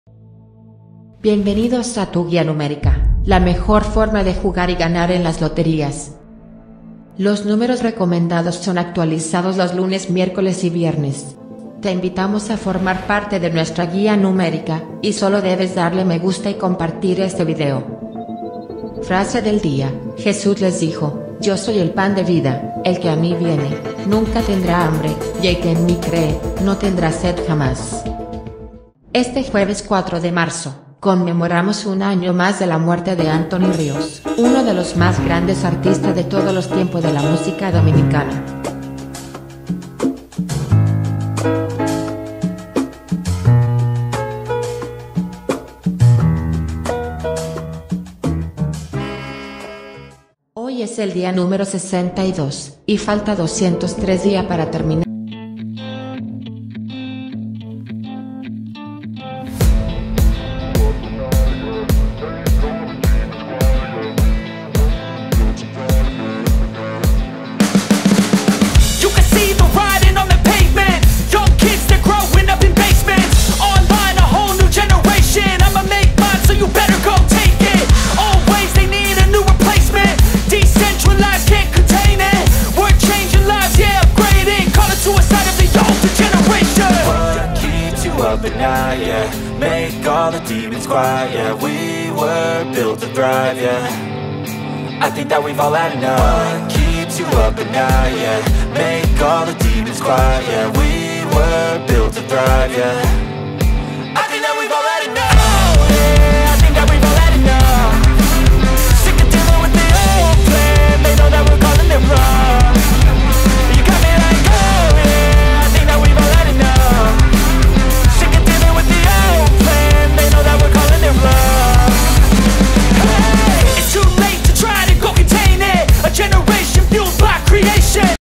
Guía Numérica - Miércoles 3 de marzo 2021 🔥🔥🔥💰💰💰
Actualizamos los mejores números recomendados para jugar en las loterías, prueba suerte con nuestros números recomendados de la Guía numérica
Números recomendados.

Línea caliente
Los números de la suerte
El pale de tu suerte
Quinielas
Lotería dominicana.